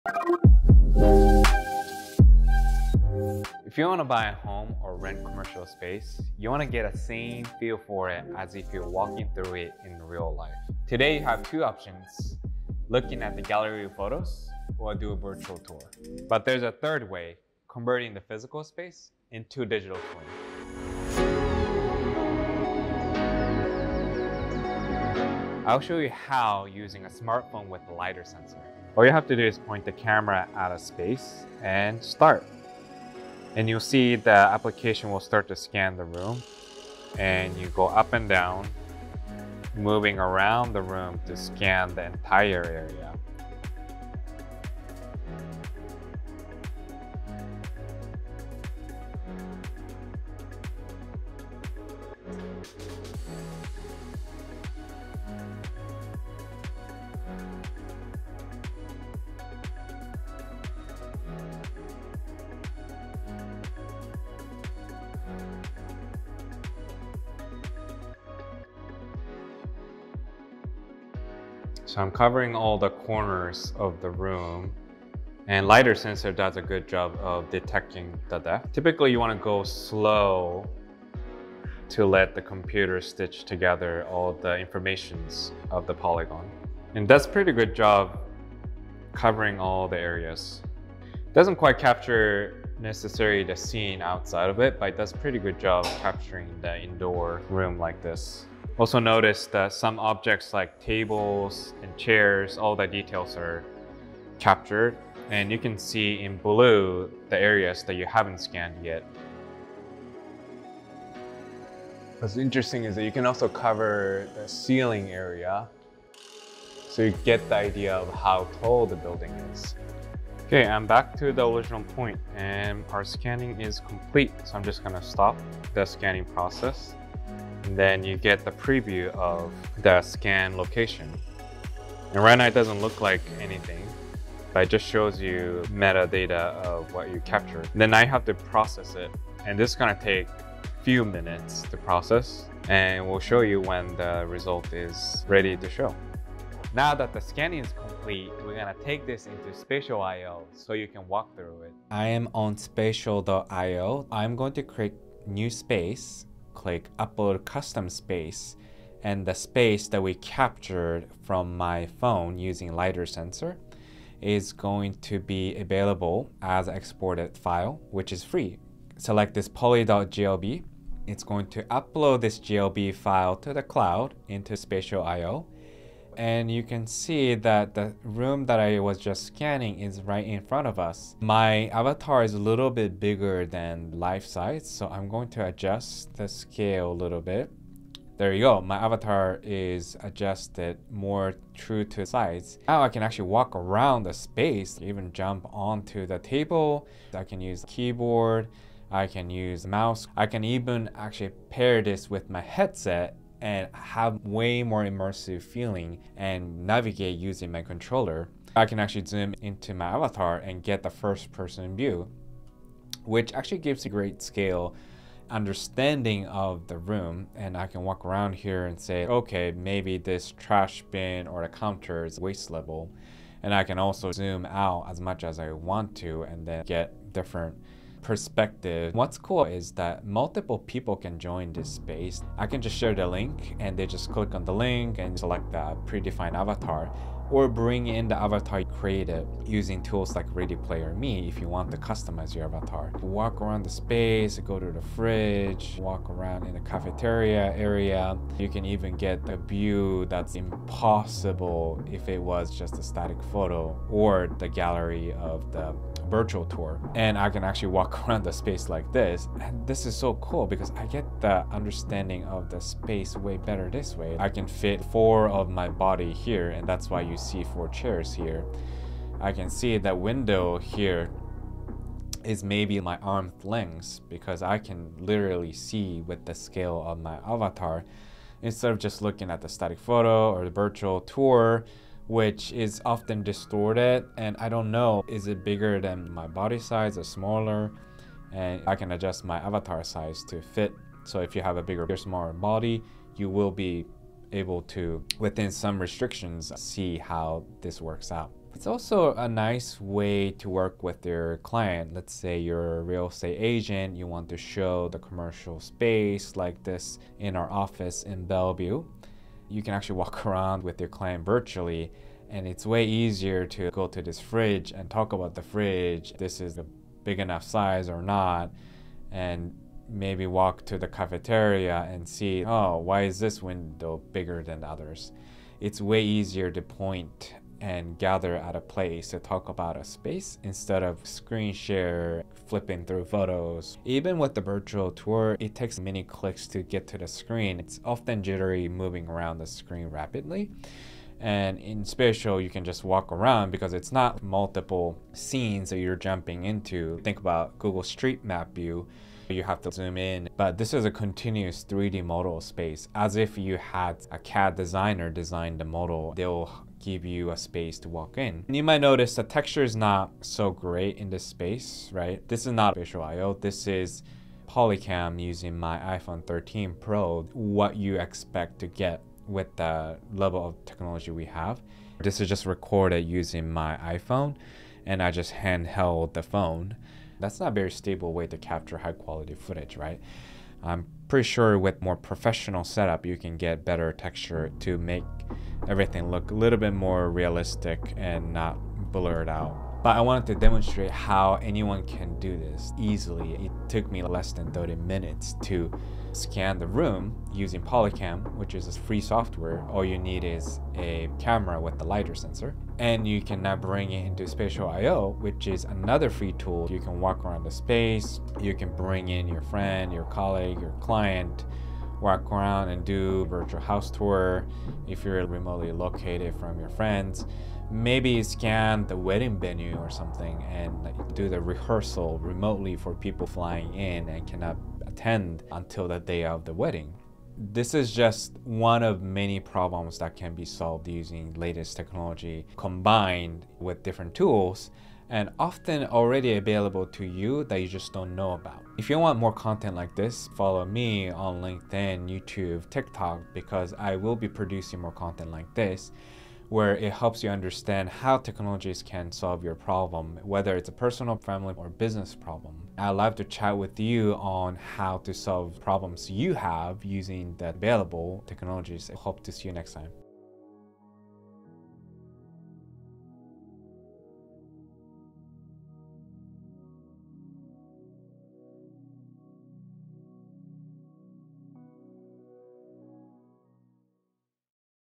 Create a Digital Twin of a Physical Space Using Only Your Phone, Polycam, & Spatial.io
The full process to create a digitaltwin of any space you want in 3 easy steps!

1) Download Polycam. Just double-check that your phone has a LiDAR sensor. Then, allow the app to help guide you capture the space directly through your phone camera 📱

3) Walk through with your avatar and invite others to join you! 🥽🏘️🏢

The ease of making a virtual space that you can walk clients through as a realtor or as a business owner looking to expand your office even when you're not there in person is the future. Try it and let me know how it goes!

‎‍ FOLLOW ELISHA ⇨ who is Fresh Consulting’s Technical Innovation Director on LinkedIn